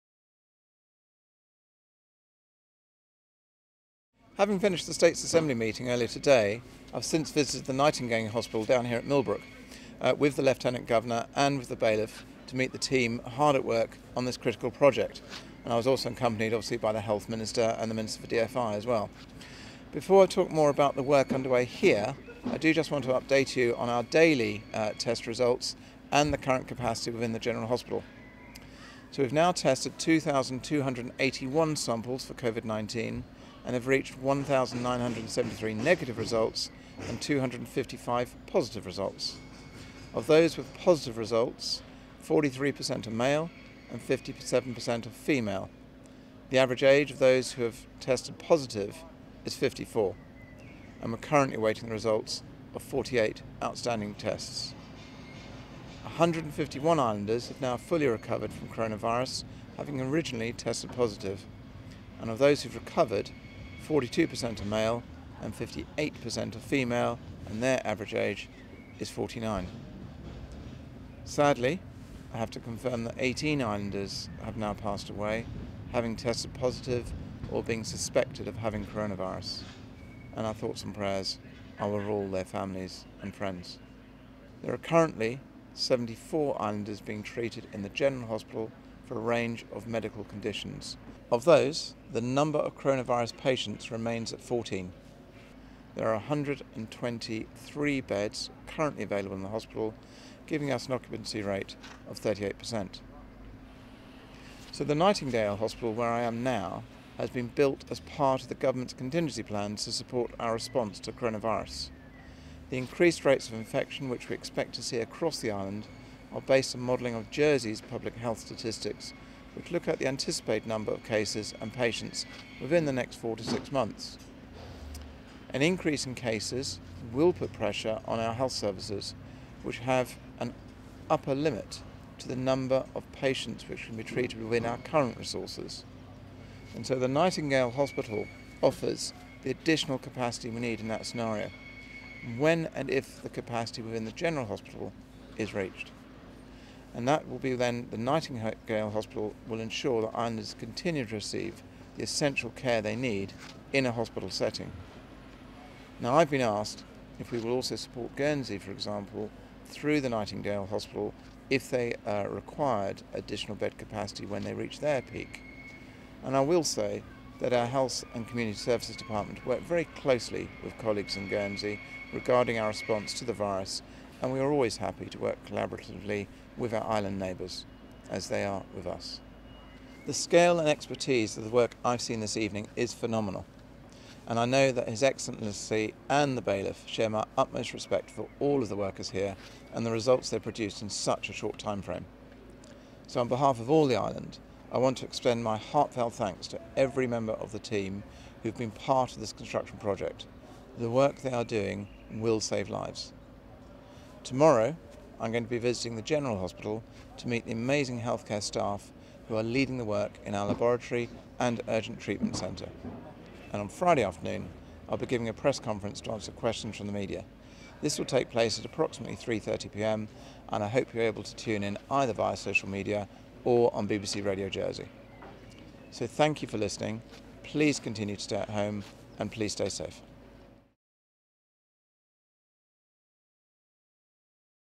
Update: Chief Minister, Senator John Le Fondré on testing and progress at the Nightingale hospital
Chief Minister, Senator John Le Fondré, provides an update on testing and progress at the Nightingale hospital following his site visit with the Lieutenant Governor and the Bailiff